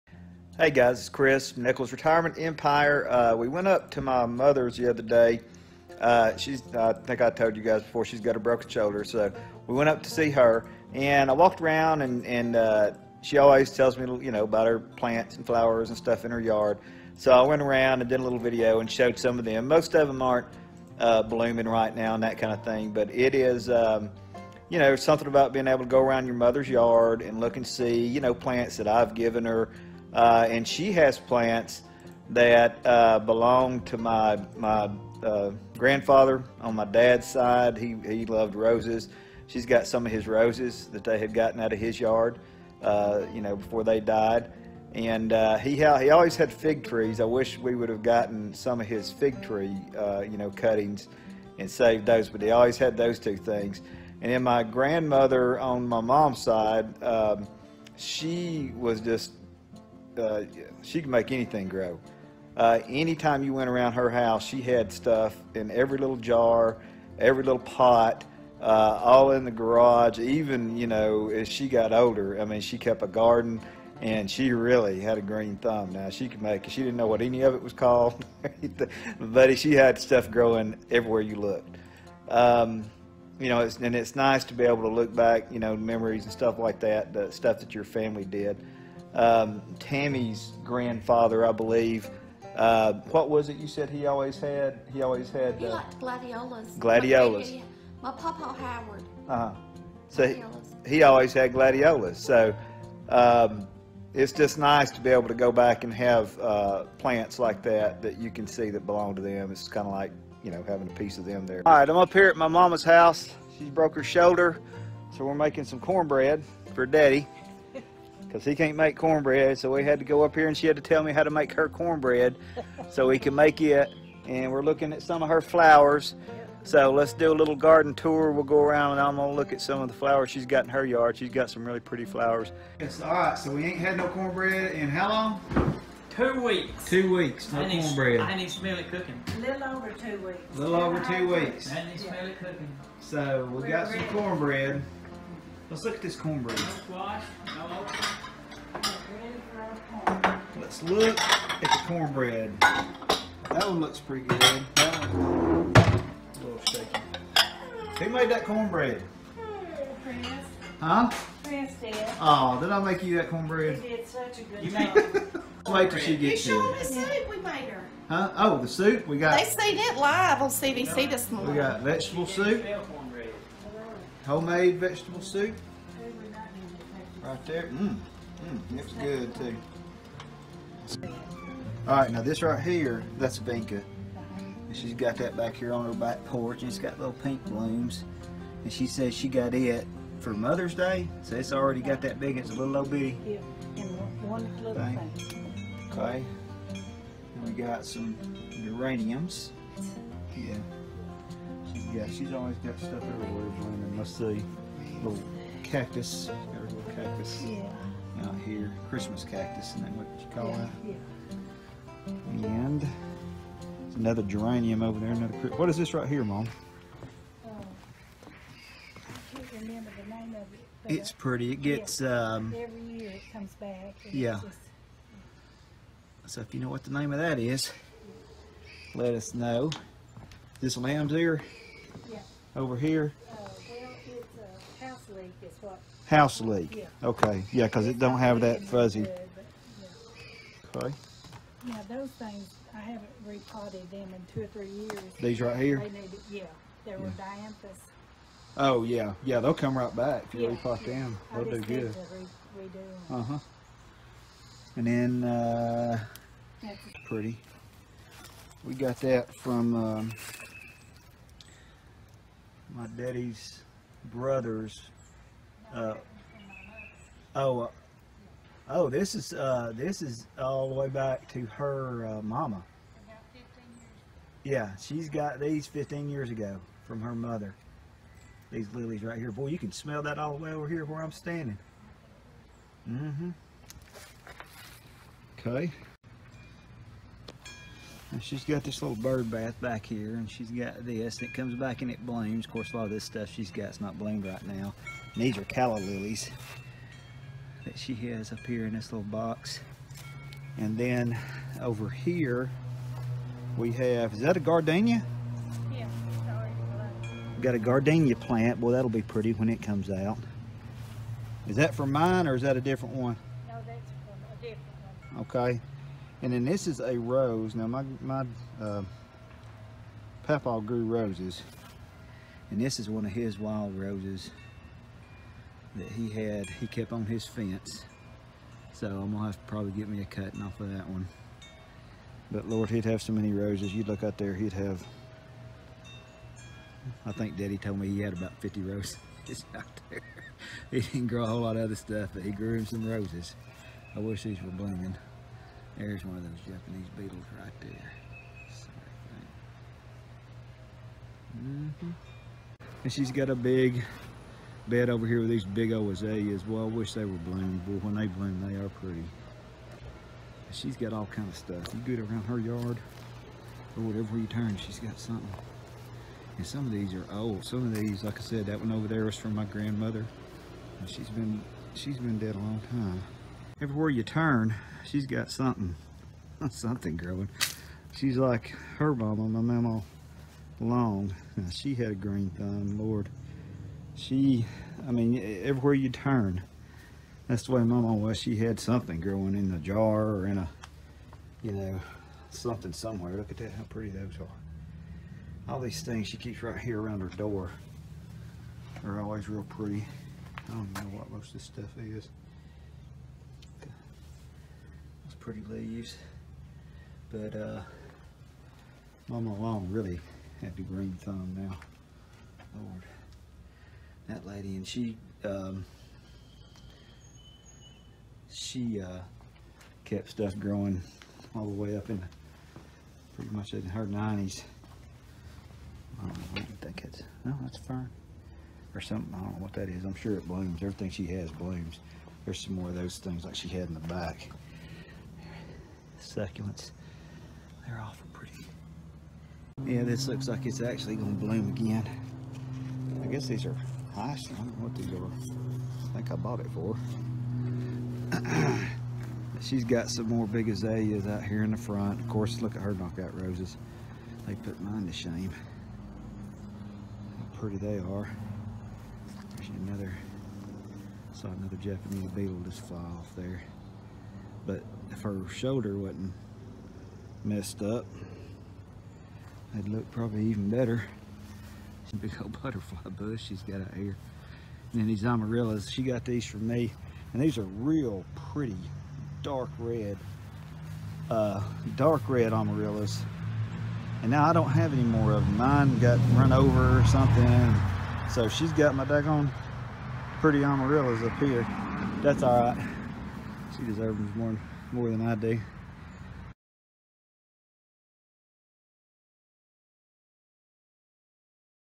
Garden Tour at Mama's House in Chickamauga-Nichols Retirement Empire
Garden Tour at Mama's House in Chickamauga-Nichols Retirement Empire. 7-9-18 I went up to my Mama's house to see her since she has broken her shoulder. We made some soup that we took to her and made her and Daddy some cornbread. Since I was there I did a short garden tour of her yard and got to see some plants that belonged to my grandparents.